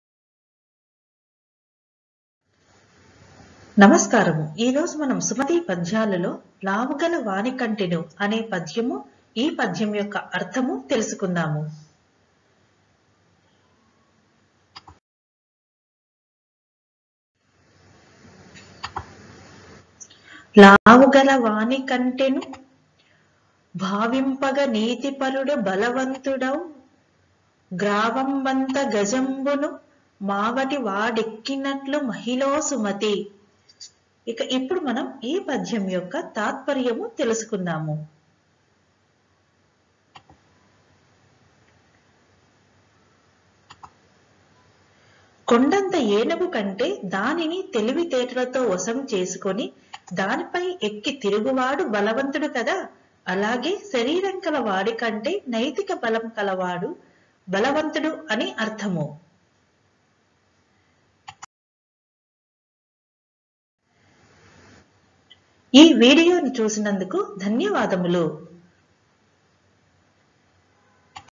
Teta Telugu - Sumati Shataka Poem with meaning - Lavugalavaani kantenu
This video shows Sumati Shataka Poem  - Lavugalavaani kantenu along its meaning also.Enjoy and learn  best poems with meaning in Telugu Language.